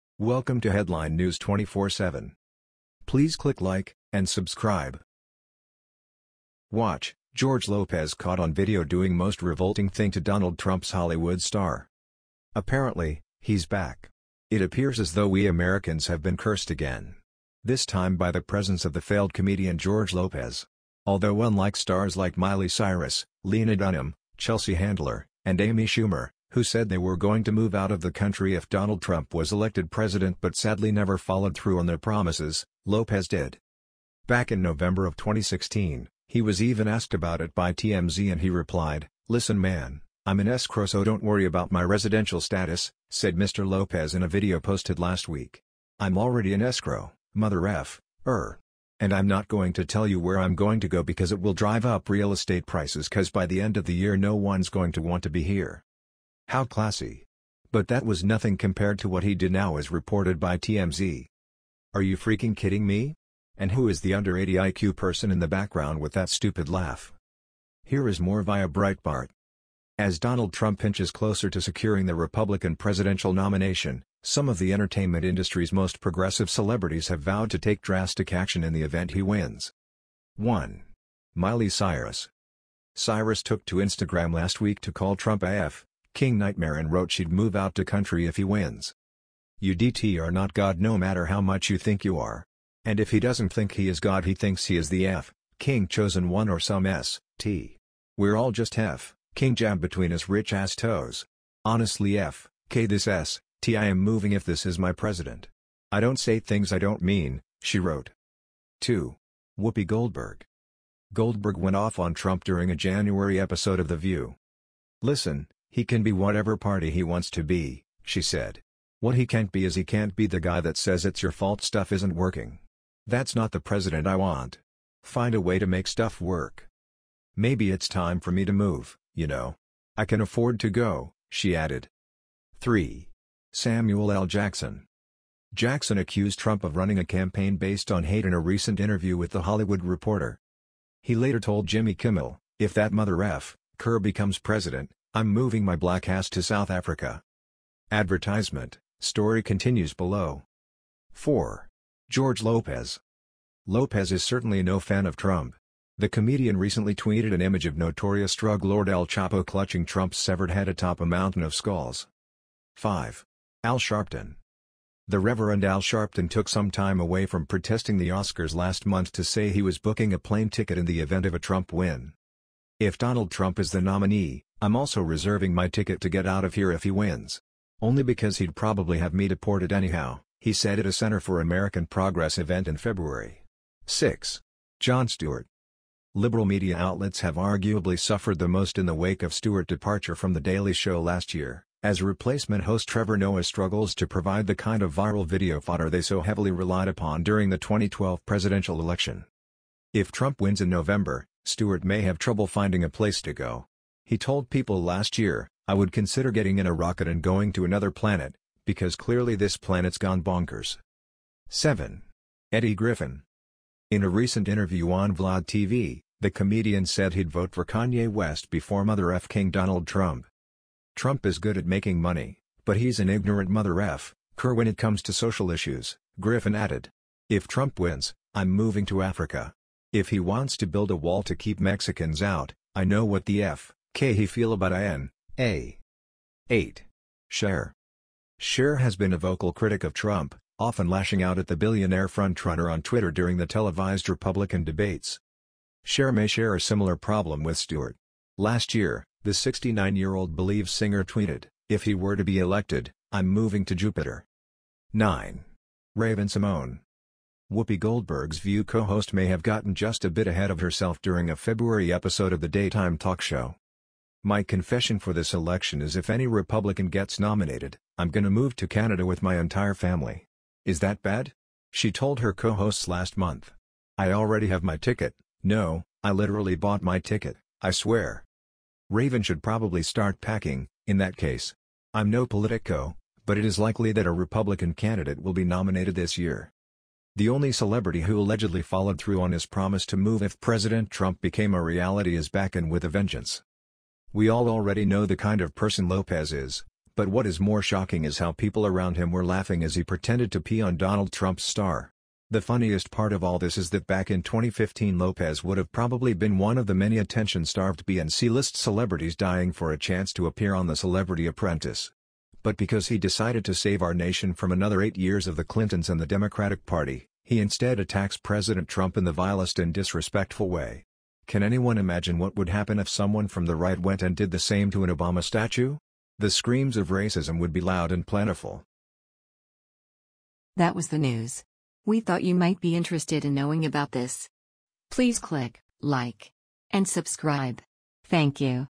WATCH: George Lopez Caught On Video Doing Most REVOLTING Thing To Donald Trump’s Hollywood Star
We all already know the kind of person Lopez is, but what is more shocking is how people around him were laughing as he pretended to pee on Donald Trump’s star.